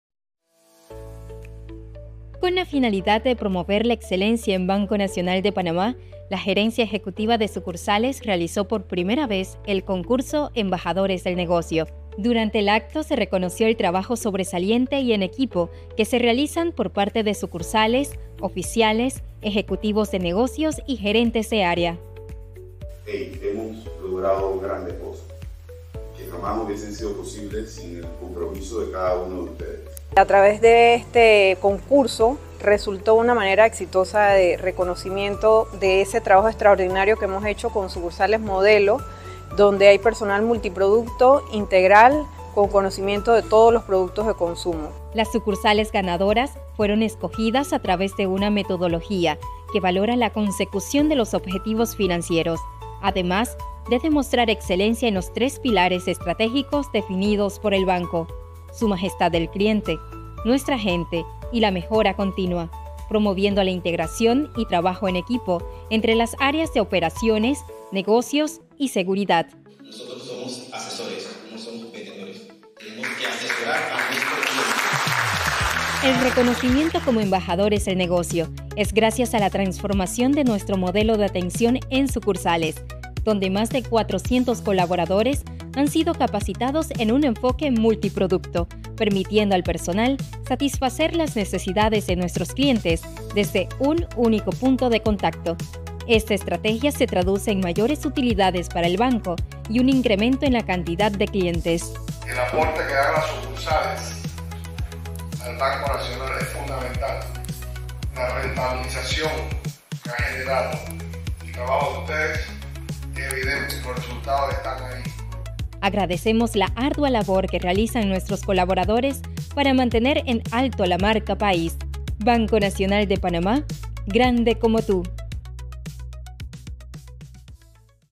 EMBAJADORES DEL NEGOCIO
Con la finalidad de promover la excelencia en el Banco Nacional de Panamá, la Gerencia Ejecutiva de Sucursales realizó por primera vez el concurso “Embajadores del Negocio”.

Los colaboradores y sucursales galardonados fueron escogidos a través de una metodología que valora la consecución de los objetivos financieros, además de demostrar excelencia en los tres pilares estratégicos definidos por el Banco: Su Majestad el Cliente, Nuestra Gente y la Mejora Continua, promoviendo la integración y trabajo en equipo entre las áreas de Negocios, Operaciones y Seguridad.